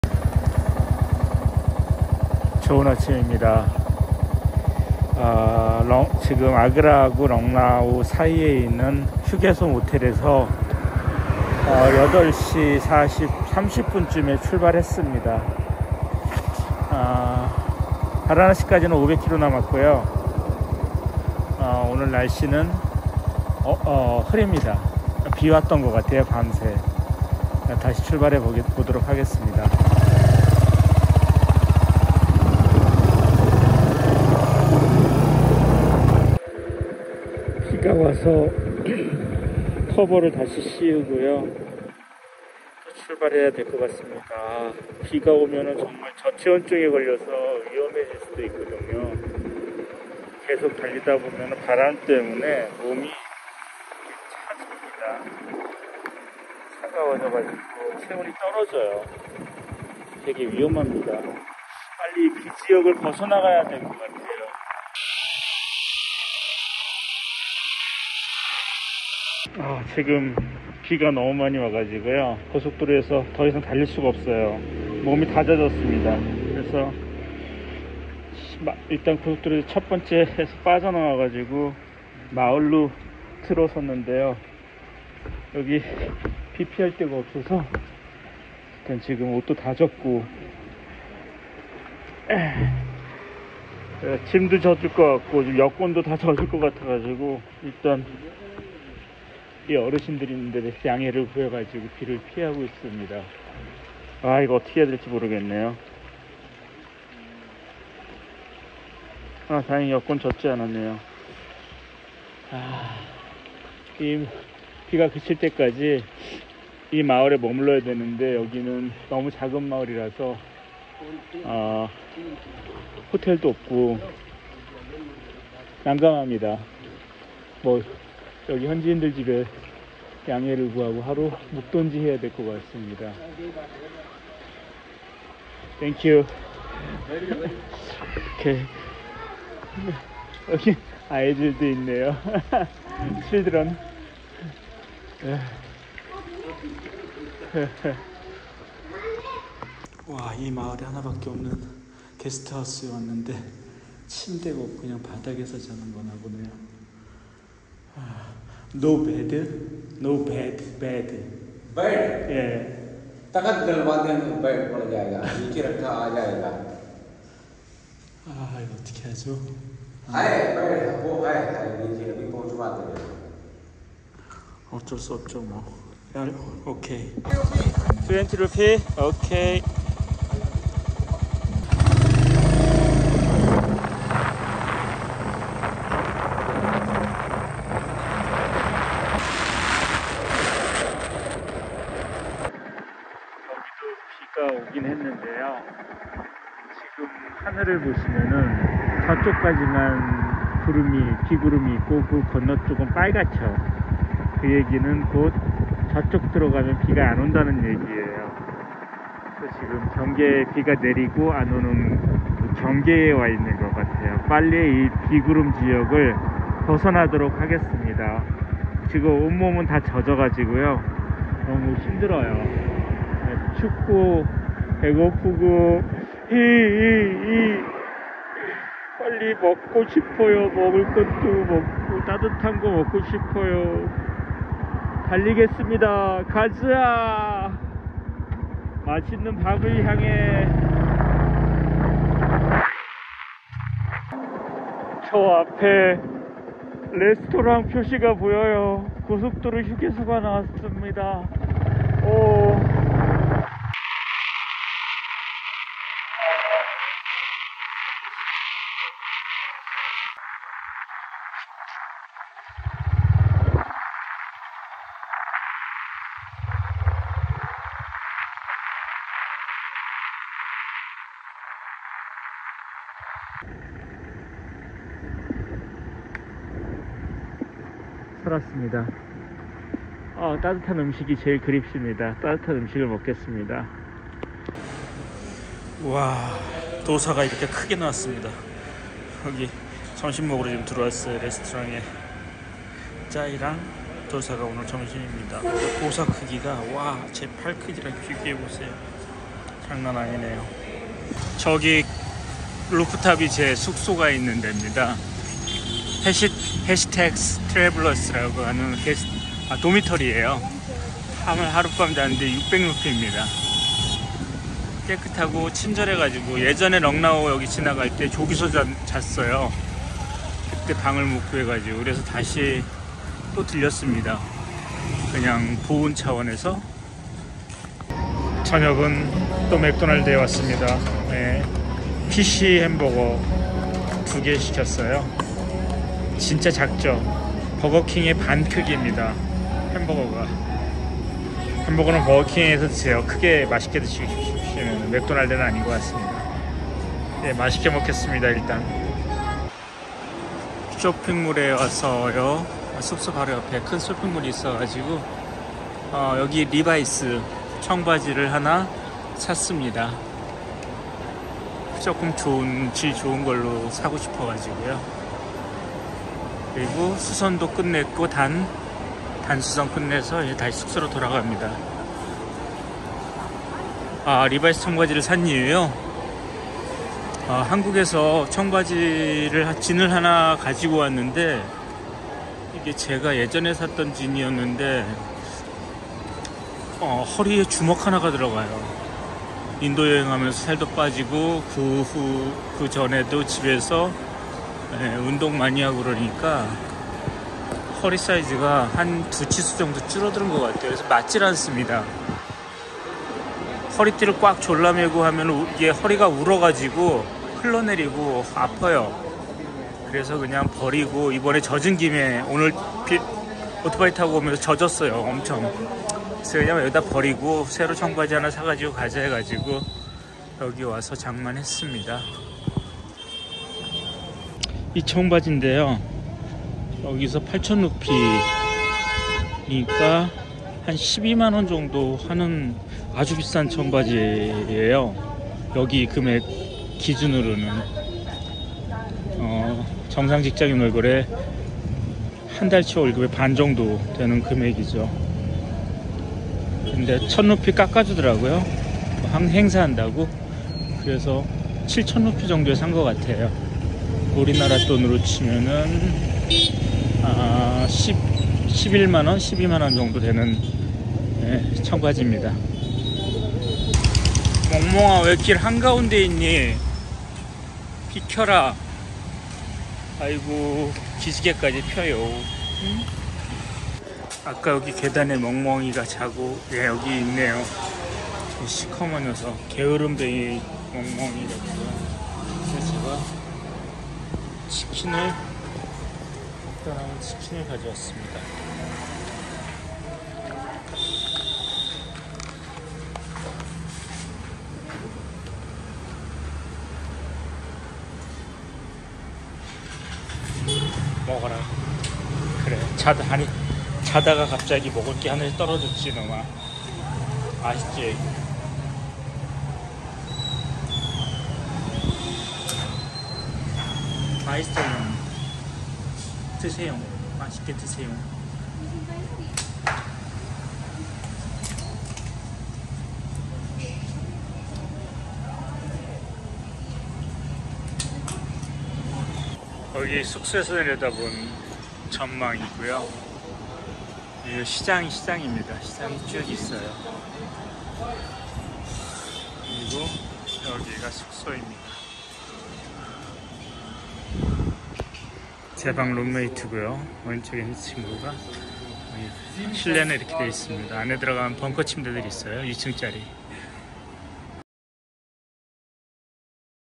D+16 Agra to Lucknow on Sep 9 바라나시 가기 전 하루 더 쉬었다 가기.
우중 라이딩 힘드러~~~무리말고 하루 쉬었다 가자